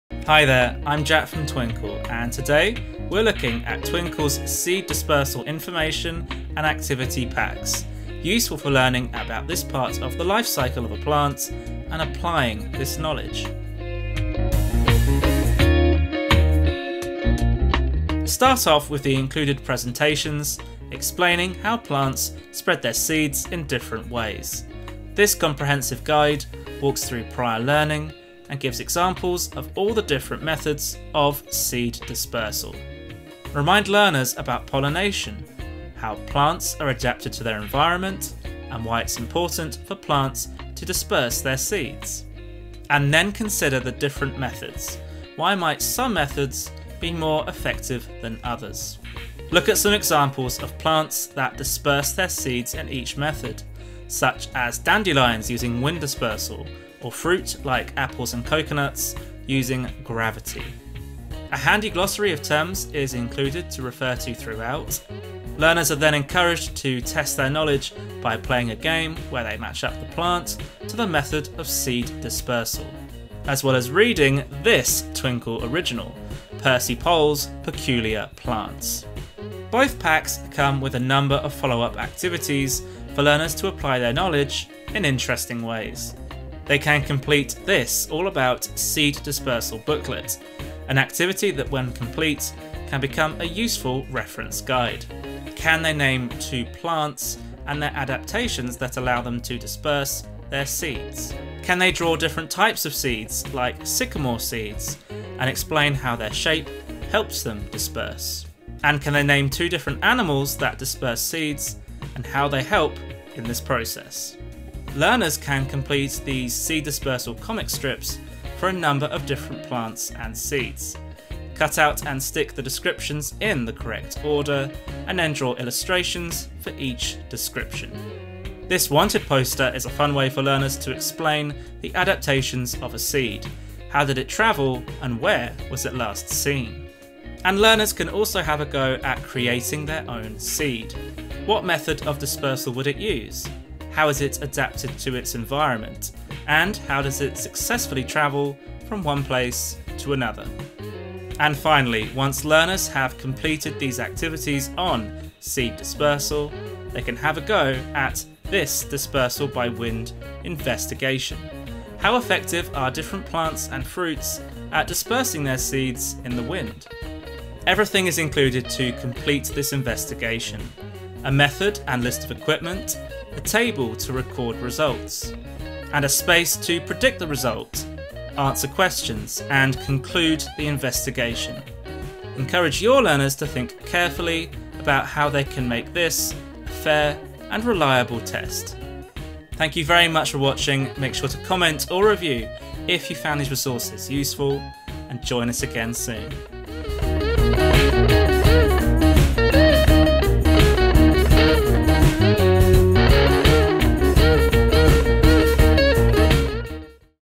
Seed Dispersal || How Do Plants Disperse Seeds
In this video, Twinkl Teacher Jack explains why it's so important that plants disperse their seeds, how they're adapted to their environment. He then showcases some science activities to use with children to learn about seed dispersal:

0:00 Introduction
0:18 How do plants spread seeds? What is seed dispersal?
1:19 Seed Dispersal Activities
2:31 Seed Dispersal Science Investigation